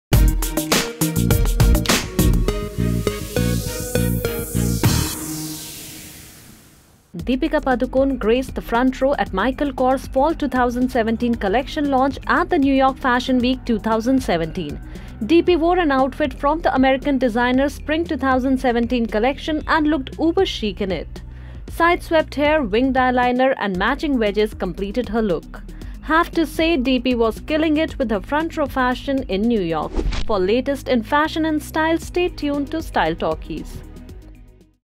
Deepika Padukone Looked Chic In Michael Kors At New York Fashion Week
Deepika Padukone graced the front row at Michael Kors' Fall 2017 collection launch at the New York Fashion Week 2017.
DeePee wore an outfit from the American designer’s Spring 2017 collection and looked uber chic in it. Side-swept hair, winged eyeliner and matching wedges completed her stylish look.